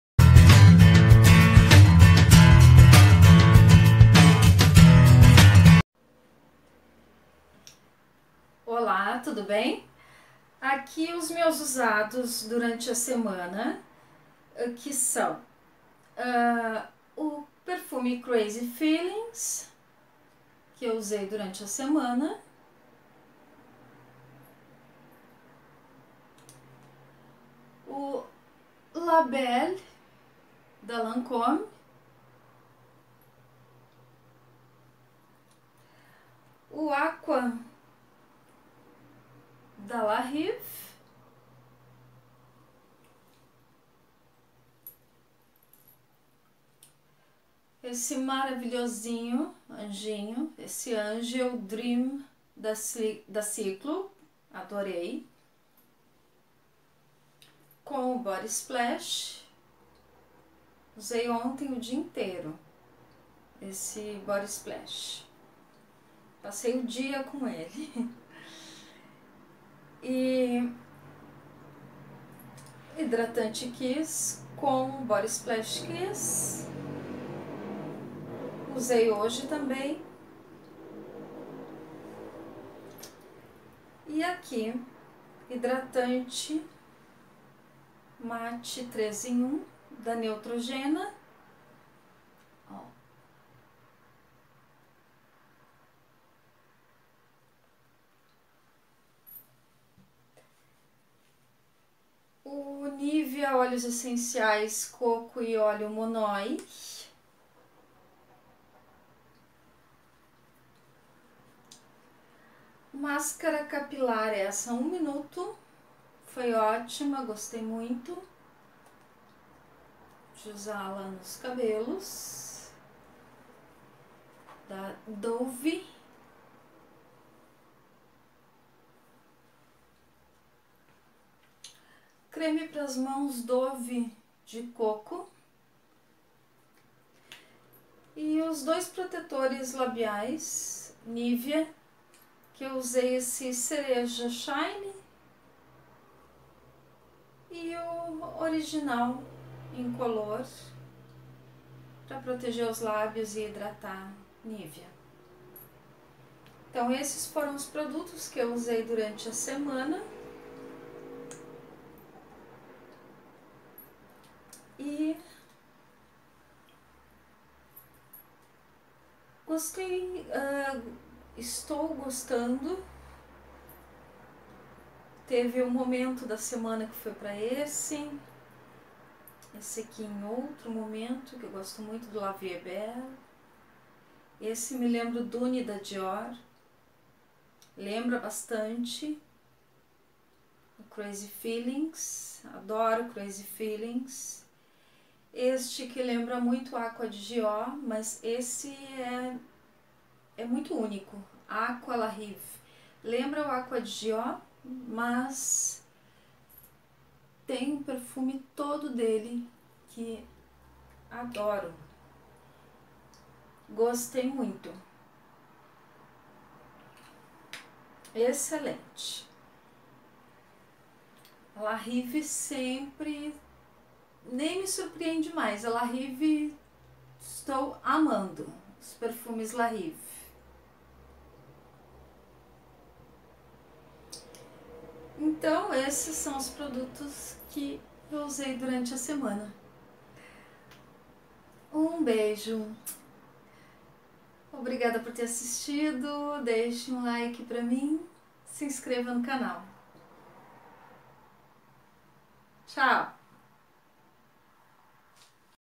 * Usados da semana *
Meus produtos usados da semana, entre cremes hidratantes e perfumes.
Compartilhando com as amigas...
Beijos,
Paty